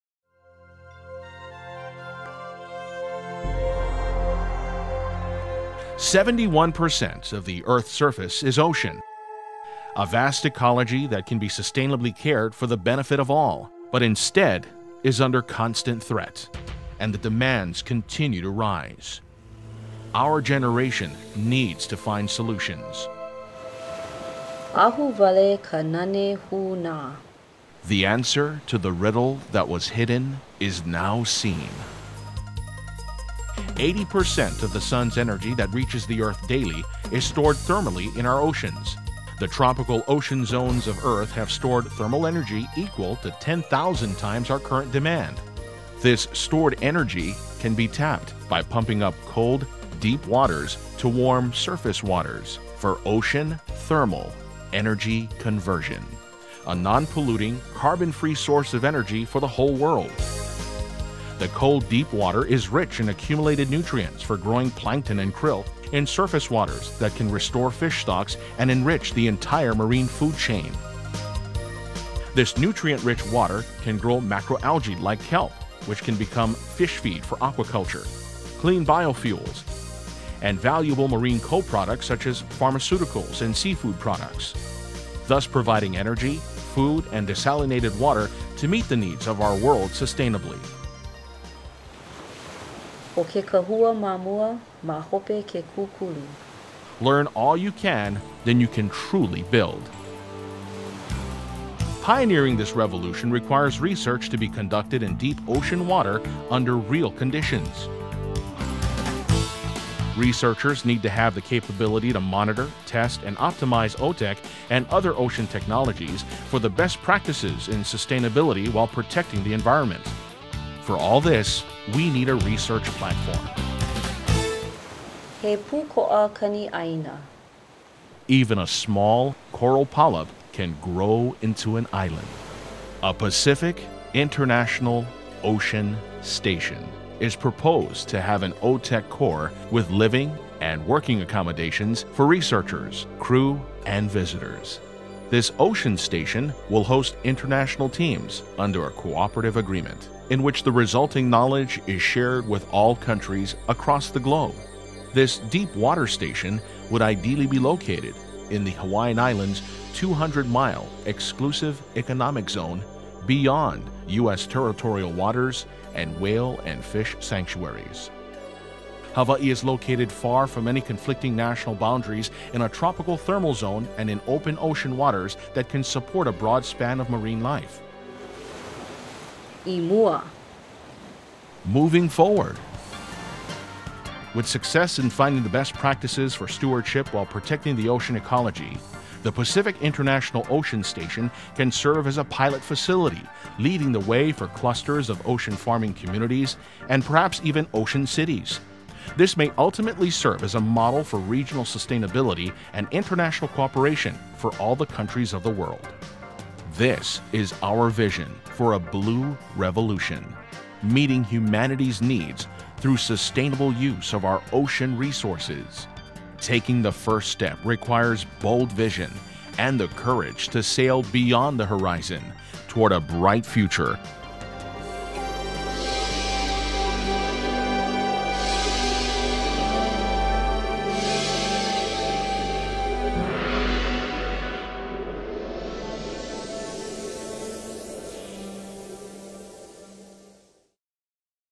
The Pacific International Ocean Station
A new organization formed in Hawaii called, Blue Revolution Hawaii advocates for the building of the Pacific International Ocean Station (PIOS) which answers the environmental, climatic, sustainable and economic feasibility of a large scale off-shore OTEC plant.  The nutrient-rich water upwelled from the OTEC process is also used for "new generation fisheries" surrounding PIOS. Blue Revolution Hawaii's board includes Patrick Takahashi (Director Emeritus, Hawaii Natural Energy Institute), Fujio Matsuda (Former President, University of Hawaii Manoa), John Farias Jr. (Former Chairman, State of Hawaii Dept of Agriculture), Leighton Chong (Patent attorney), Matt Matsunaga (Former State Senator and son of US Senator Spark Matsunaga), Guy Toyama (Executive Director, Friends of NELHA), Ka'iu Kimura (Executive Director, Imiloa Center), Gov George Ariyoshi (Former Governor, State of Hawaii).